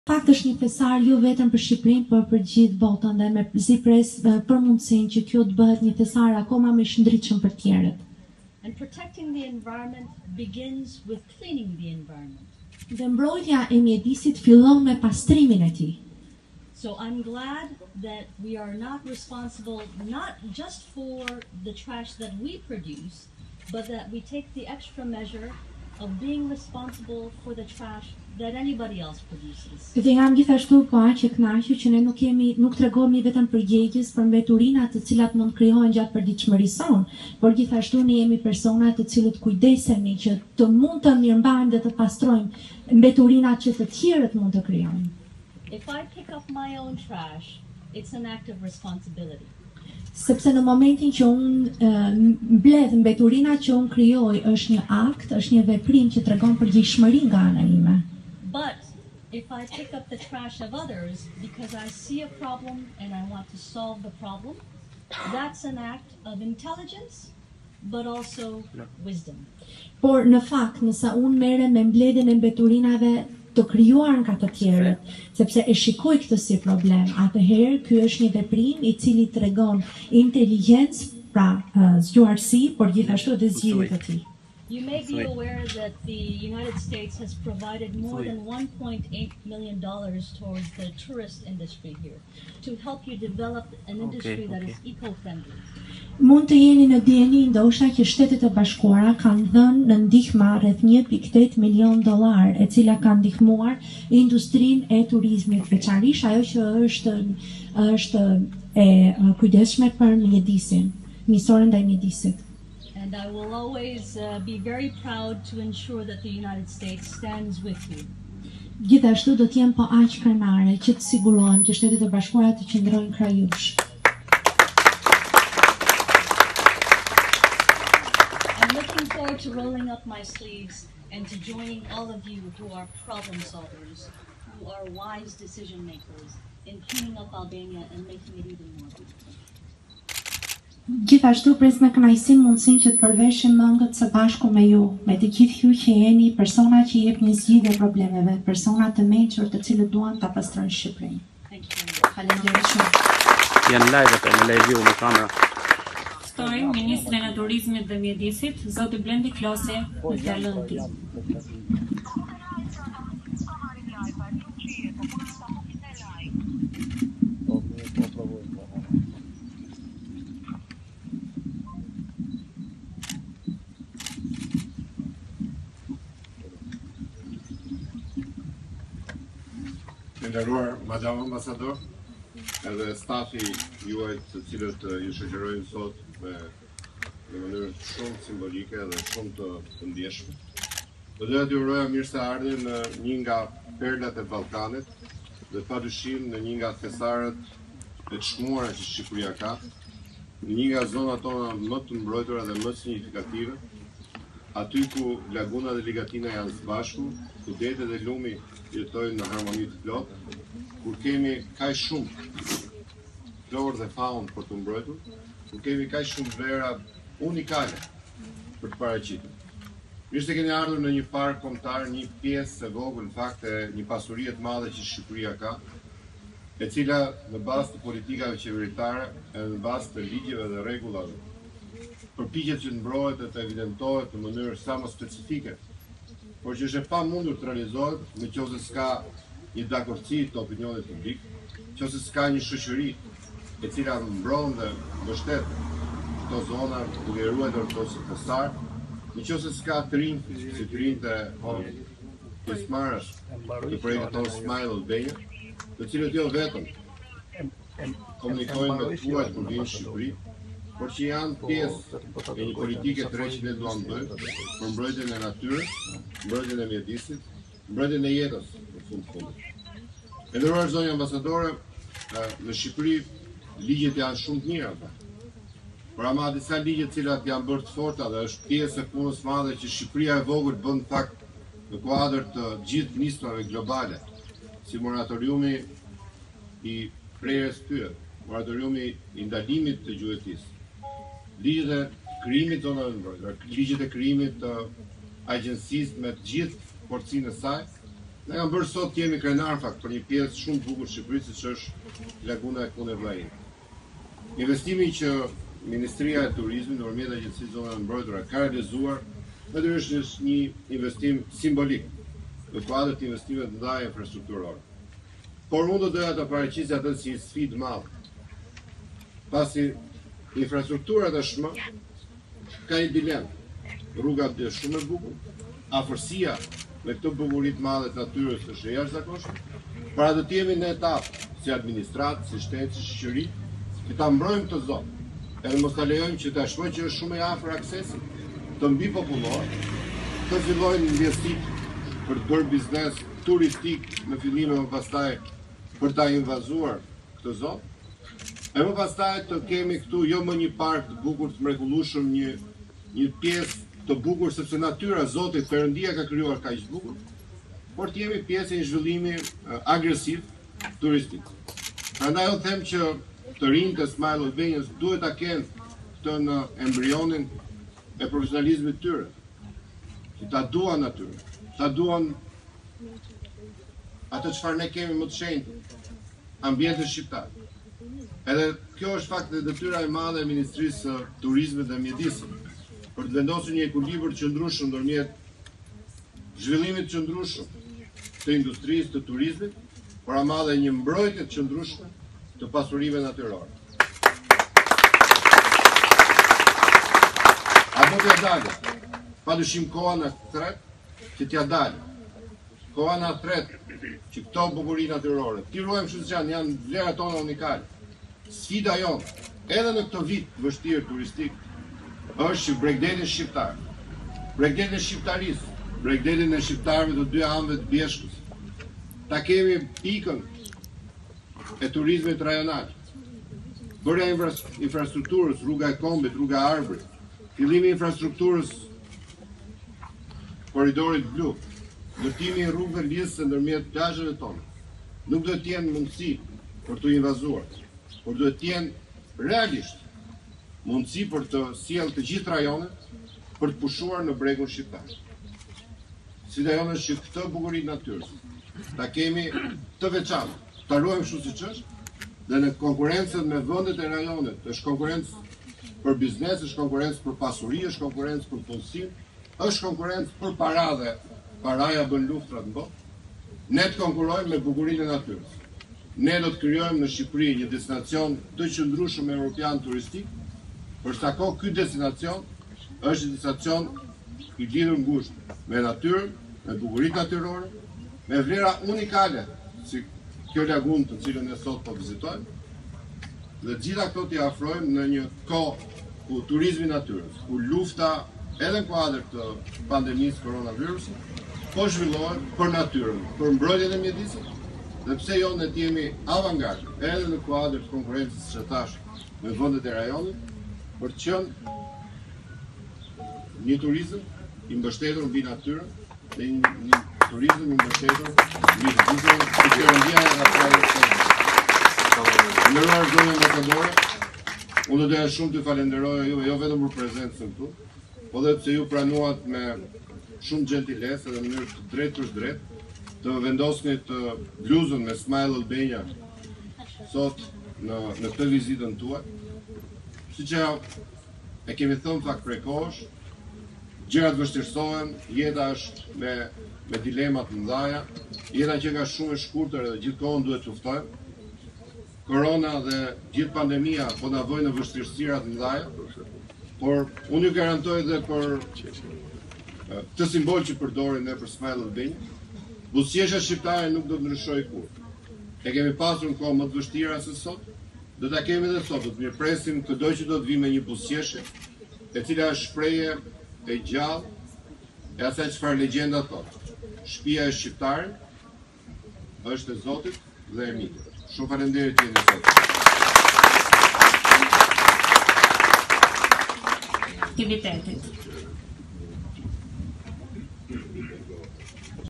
Lezhë, Ambasadorja e SHBA-së: Të përveshim mëngët, me njerëzit e mençur do zgjidhim problemet
Ambasadorja e SHBA, Yuri Kim nga Lezha ku mori pjesë së bashku me ministrin Klosi në një aktivitet për turizmin është shprehur se Amerika është në krah të Shqipërisë. "Fillimisht Mesazhi im sot për ju, Ju Falenderoj që më keni ftuar që të jem këtu me ju Eshtë një kënaqësi shumë e madhe që të jem këtu me ju. Ghithashtu kam dëgjuar nga ministri që ju jeni vullnetarët e parë të cilët mikprisni turistët në Shqipëri dhe ju I afroni përshëndetjet tuaja.

Ky vit është paksa ndryshe nga vitet e kaluara pasi ka problemin e corona virusit , por jam e kënaqur të shoh se si keni bërë përshtatje në bazë të ndryshimit të situatës . Në vend që të gjenit një mënyrë për të mos e vazhduar projektin tuaj ju keni gjetur një mënyrë tejet të mirë për të vazhduar me të. Kam tani në Shqipëri 6 muaj dhe kam pasur mundësinë që të vizitojë shumë zona të bukura. Berat, Vlora, Saranda, pasksa në Krujë.

Po jam po aq e kënaqur që kam mundësinë që të jem këtu sot në mënyrë që të dëshmoj bukurinë natyrore të Shqipërisë . sepse në fakt është një thesar jo vetëm për Shqipërinë por për gjithë botën dhe me mezi pres që kjo të bëhet një thesar akoma më i shndrritshëm për të tjerët. Dhe mbrojtja e mjedisit fillon me pastrimin e tij.

Dhe jam po ashtu po aq e kënaqur që je nuk jemi, nuk tregohemi vetëm përgjegjës për mbeturinat të cilat mund të krijohen në përditshmërinë tonë por gjithashtu ne jemi persona, të cilët kudjesemi që mund ta mirëmbajmë dhe ta pastrojmë mbeturinat që të tjerët mund të krijojnë sepse në momentin që unë mbledh mbeturinat që unë krijoj është një akt që tregon përgjegjshmëri nga ana ime, por në fakt ndërsa unë merrem me mbledhjen e mbeturinave të krijuara nga të tjerët sepse e shikoj këtë si problem.